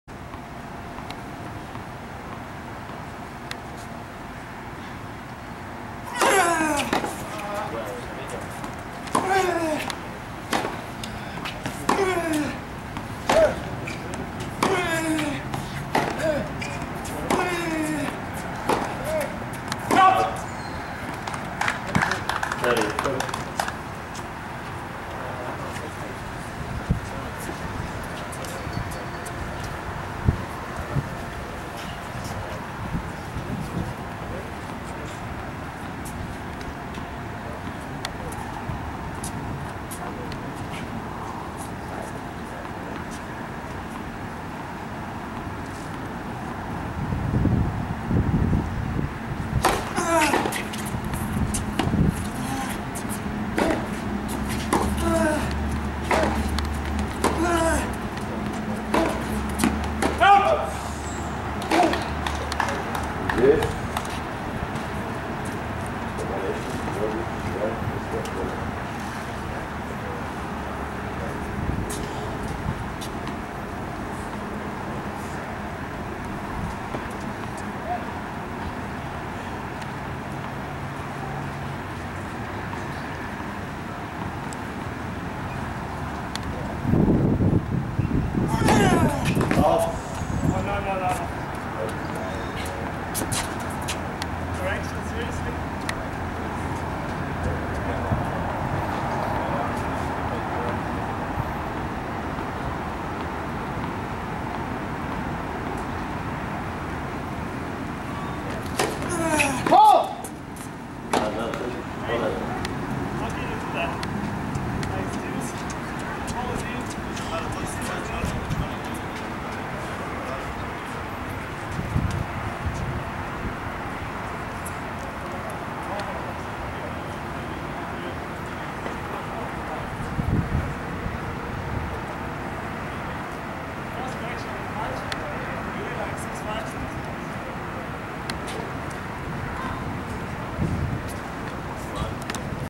Match Point With Illya Marchenko And Simone Bolelli!
3-11-14:
First of three matches I kept an eye on this day. These two gents has some good volleys and I think I got a good sequence this instance. Becoming more and more of a fan of HD recordings. Enjoy Tennis fans. More to come this weekend!